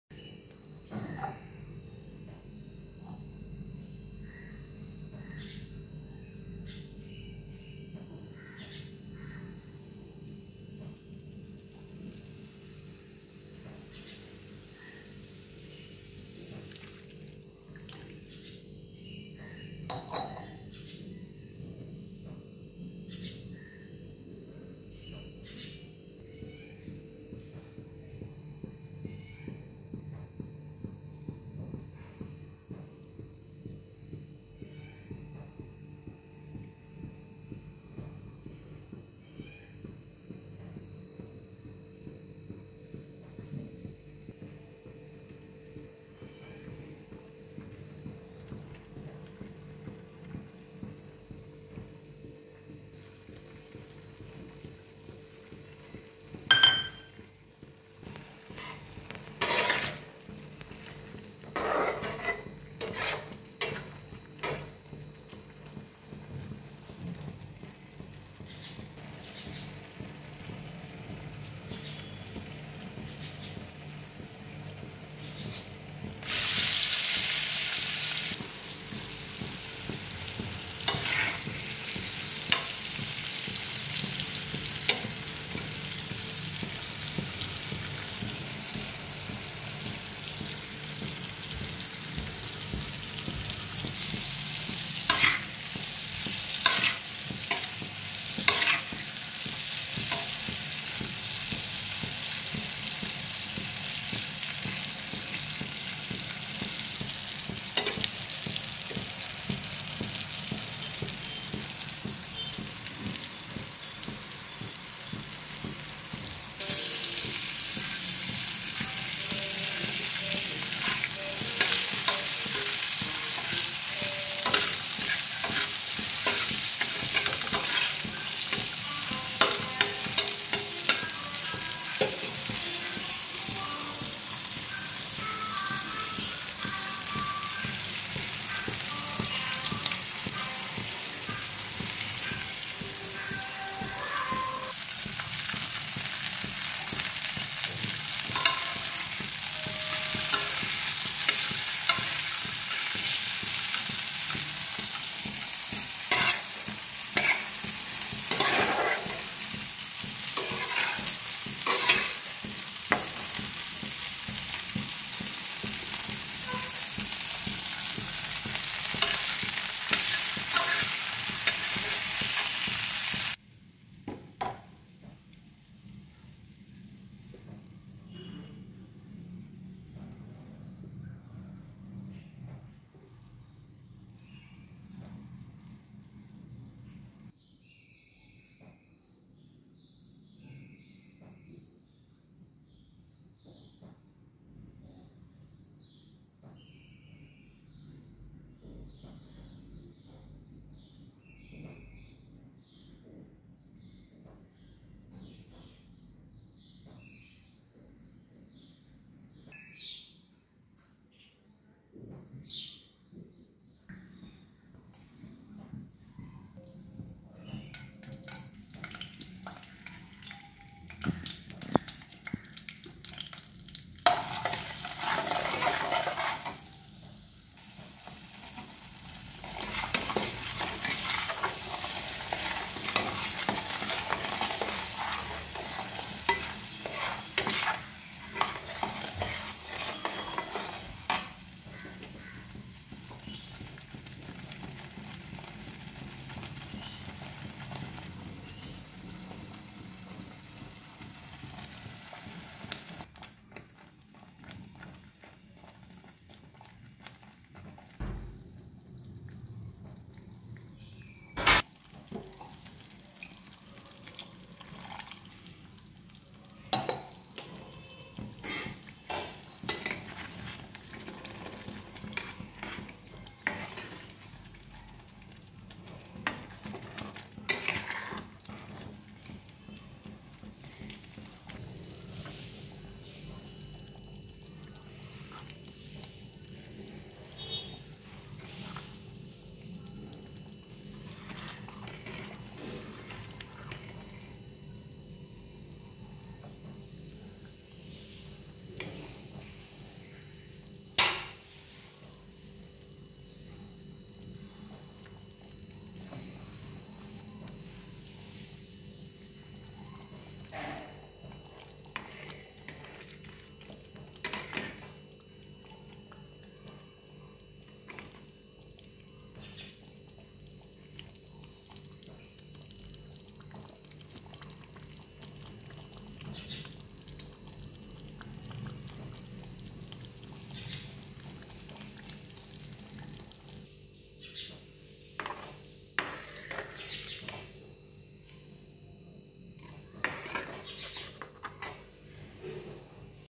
पनीर रेसिपी । how to make easy paneer recipe restaurant style
Paneer recipe restaurant style very easy and tasty